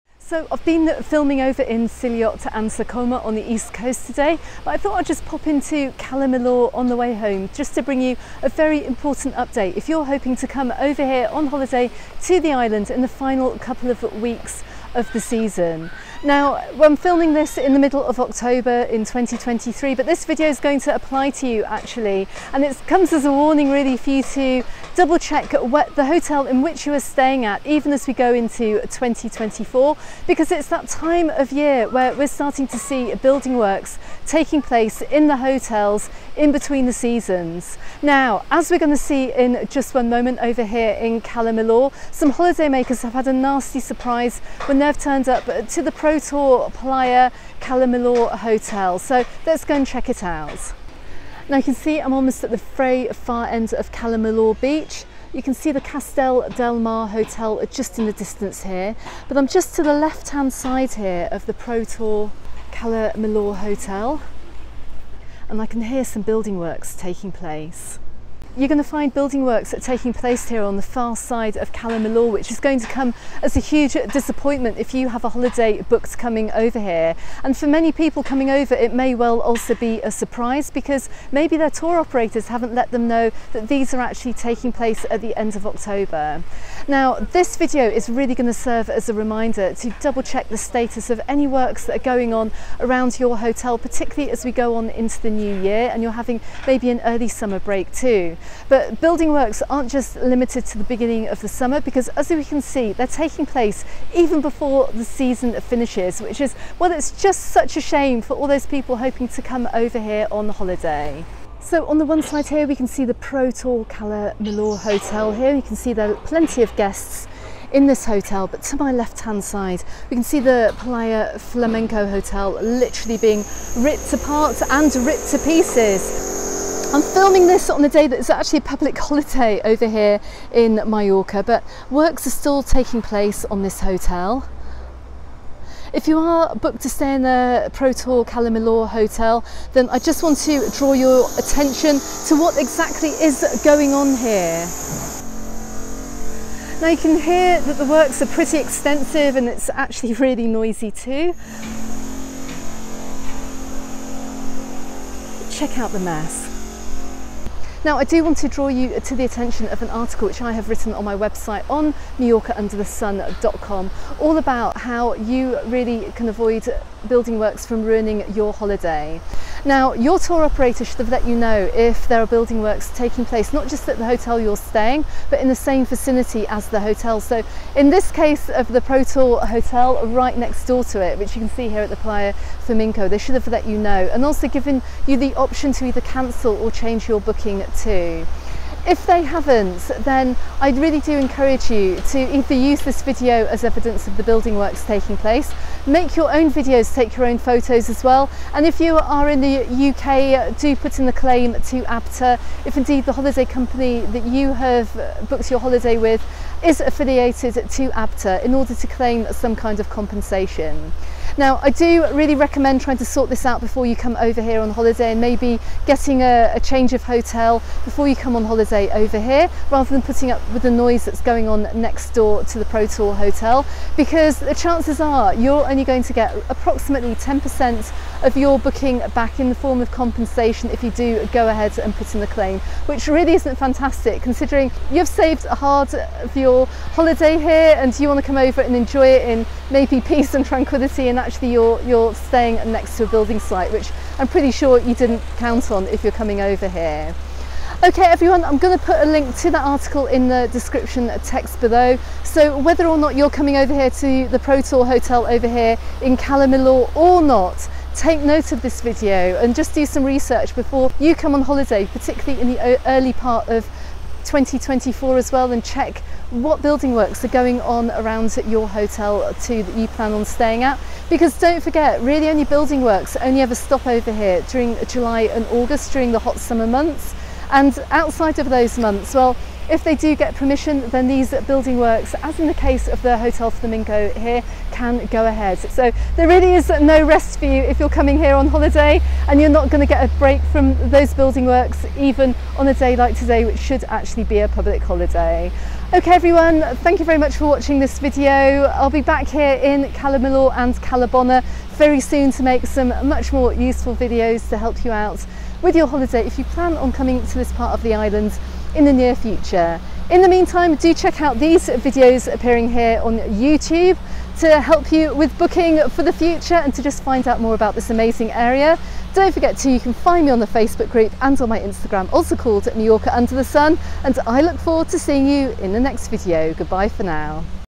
⚠️WARNING for your next Mallorca Holiday | A nasty surprise in Cala Millor
In this video, I have a WARNING for your next Mallorca holiday following a nasty surprise experienced by some holidaymakers who booked the Protur Playa Cala Millor Hotel in October 2023.

To find out more about what to do if your holiday is affected by building work, and how to avoid this happening, check an extensive article I wrote on this topic here:

You can also read the Trip Advisor reviews of the Protur Playa Cala Millor Hotel in October 2023